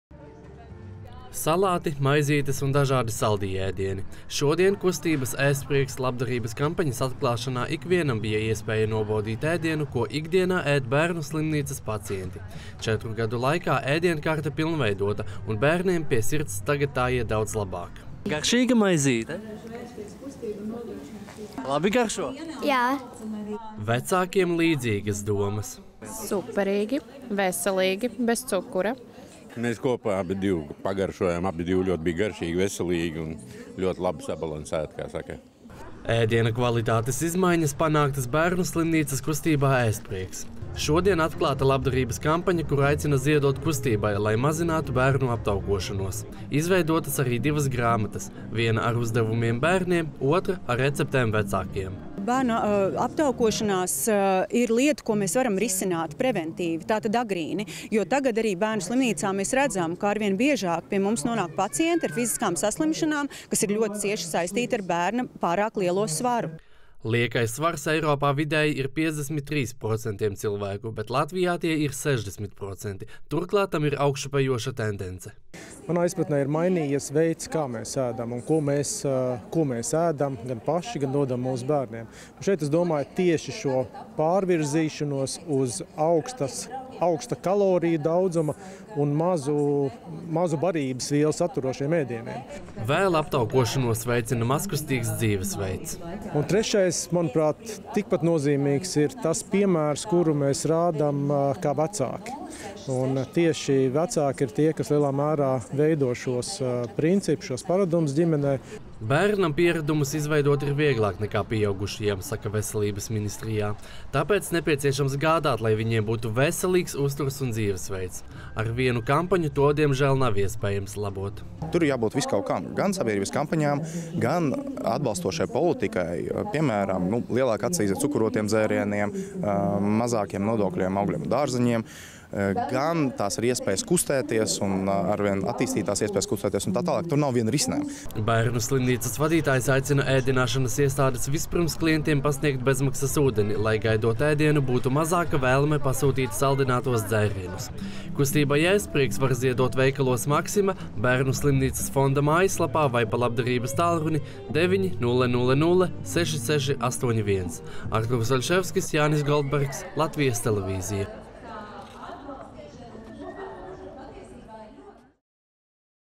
Aicina ziedot aptaukošanās mazināšanas kustībai
Lai Bērnu slimnīcas pacientiem turpinātu uzlabot ēdienkarti un vispārīgi mazinātu bērnu aptaukošanos, atklāta kustības “Ēstprieks” labdarības kampaņa, kurā ziedot aicināts ikviens. Latvijā pieaug liekā svara problēma un situācija ir sliktāka nekā Eiropā vidēji. Veselības ministrija ir izstrādājusi plānu, kā vismaz mazināt problēmas pieaugumu.